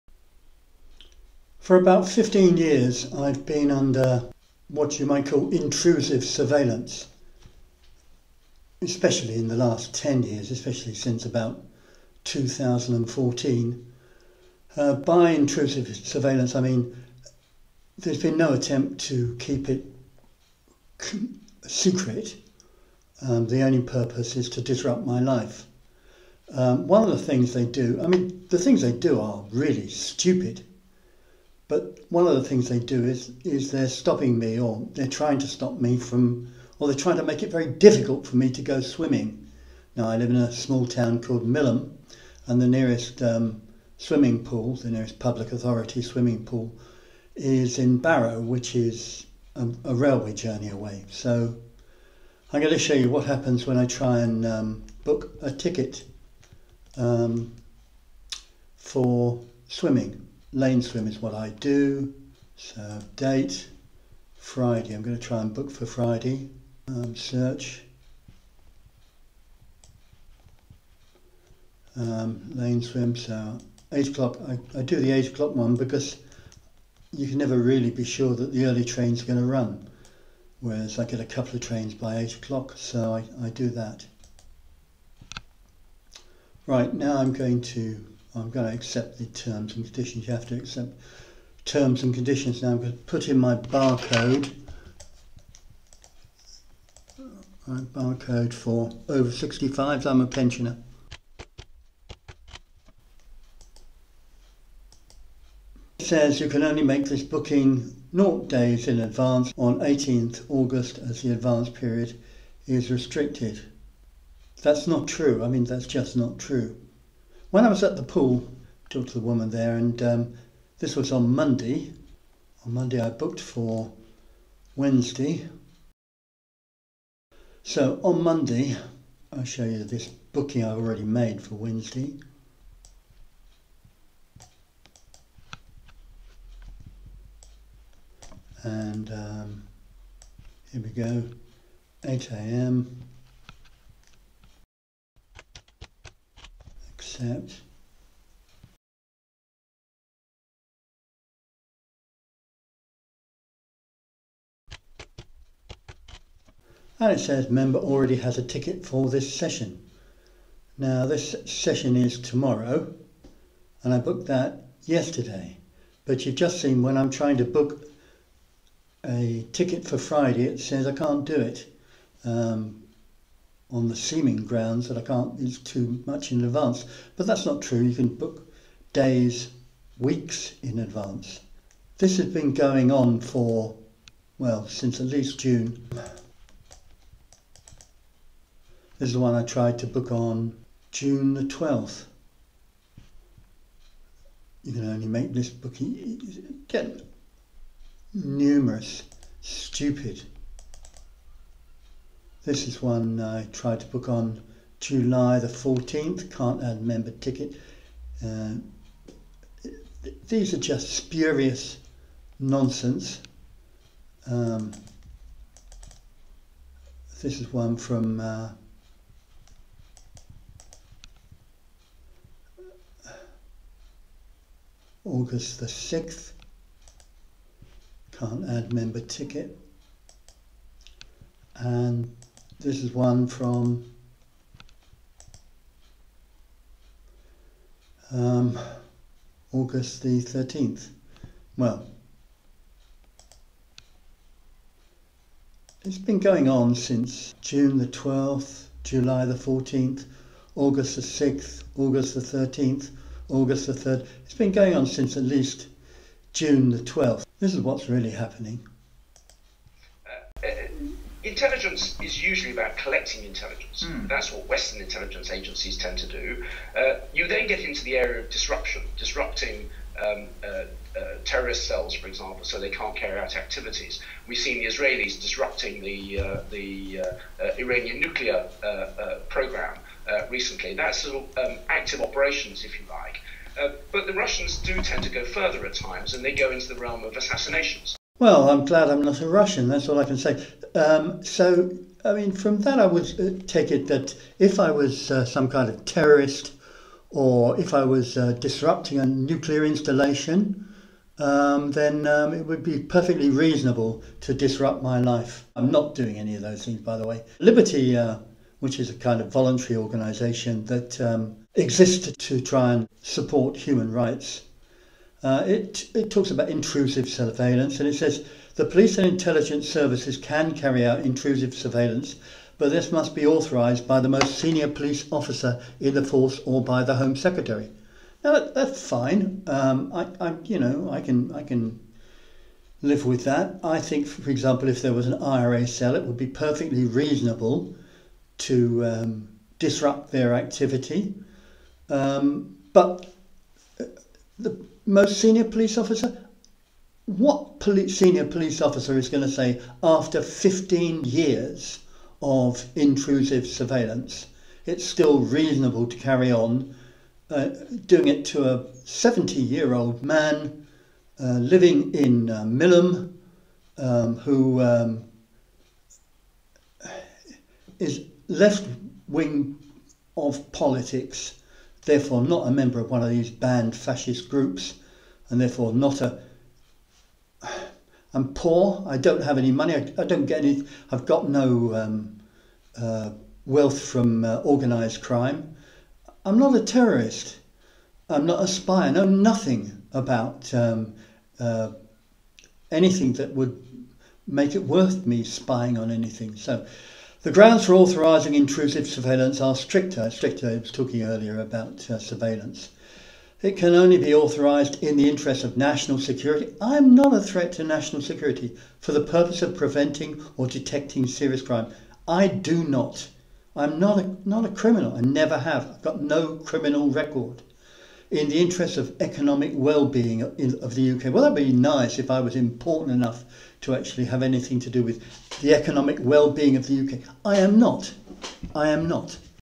Spooks Attempt to Disrupt Swim
People often want to know what they are NOT supposed to know and the deception agencies (MI5, MI6, GCHQ etc) went to a great deal of trouble to stop you seeing this. The subject is harassment by the state. I would call it torture or extra judicial punishment. It involves small almost insignificant actions, but the cumulative effect after many years is devastating.